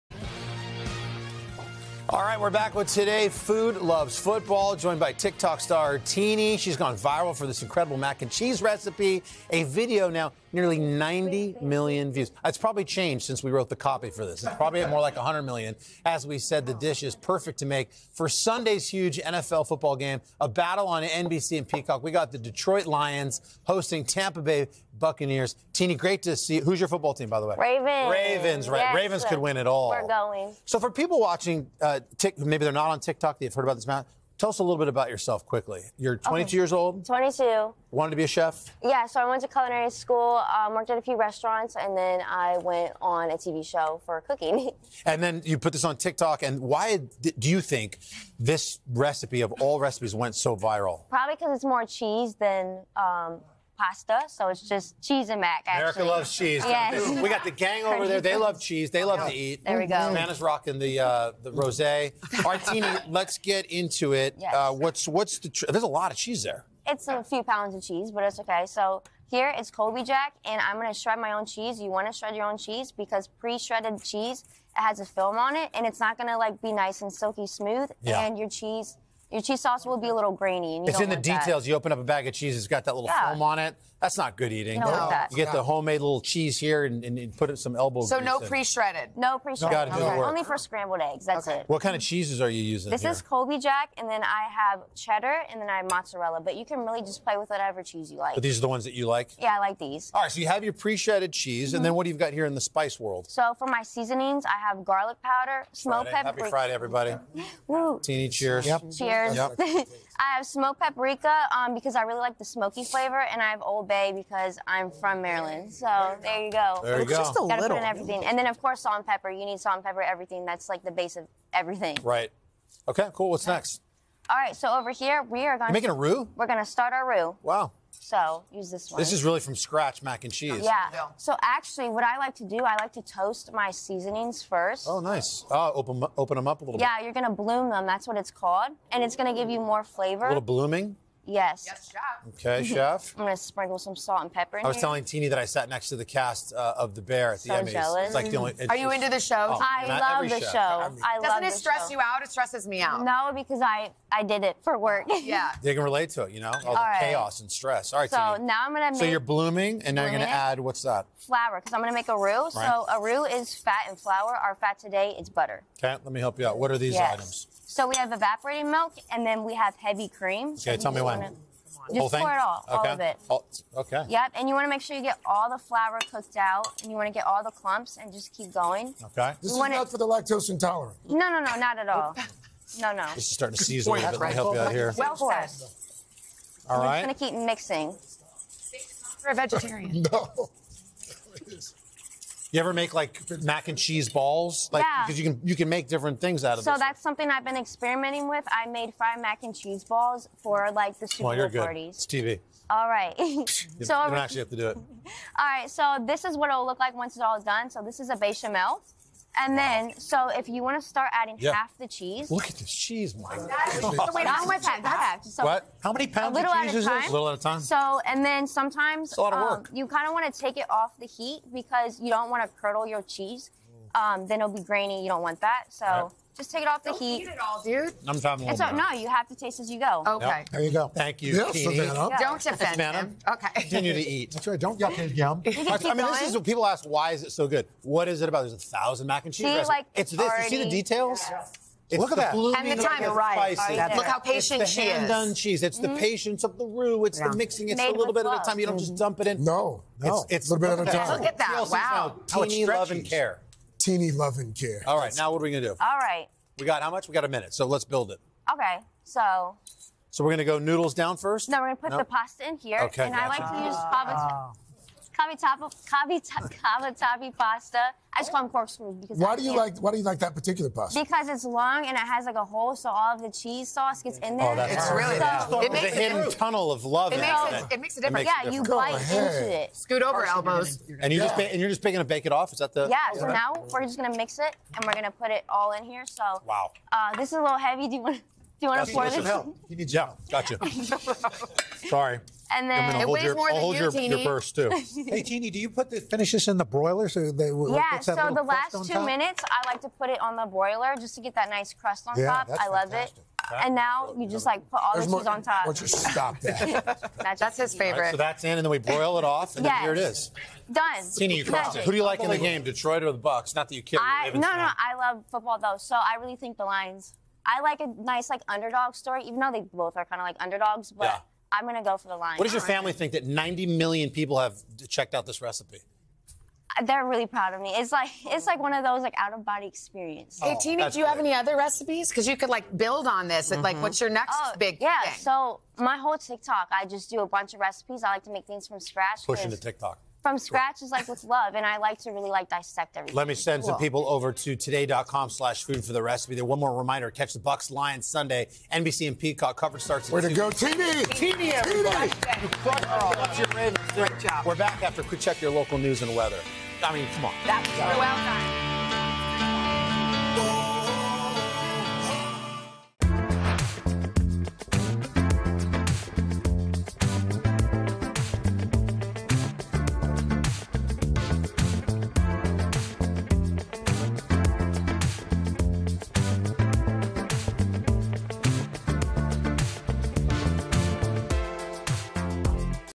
Get TikTok star Tini’s viral macaroni and cheese recipe
TikTok star Tini stops by Studio 1A to share her viral macaroni and cheese recipe and says that shredding your own cheese will make for a silkier cheese sauce.